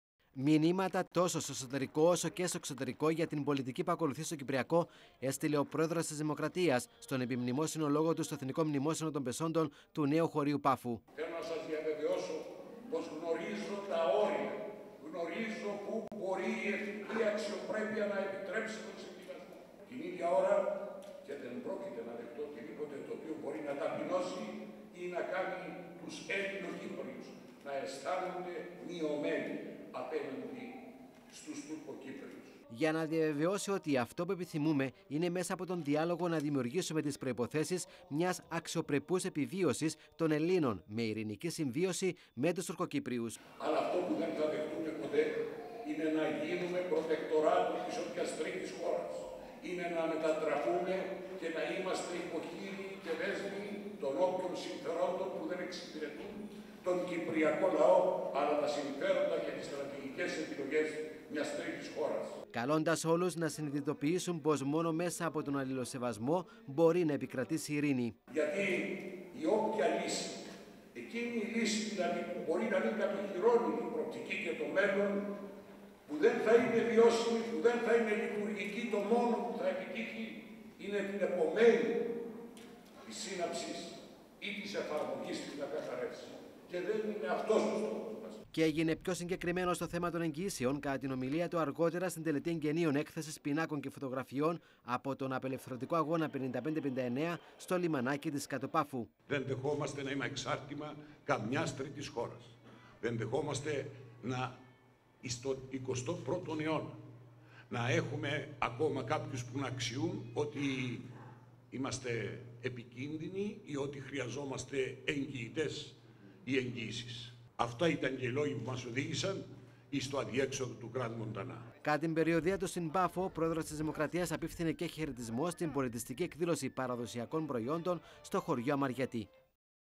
Πρόεδρος: "Δεν θα δεχθώ οτιδήποτε που θα ταπεινώνει τους Ελληνοκύπριους"
Ξεκάθαρα μηνύματα Προέδρου Αναστασιάδη προς κάθε κατεύθυνση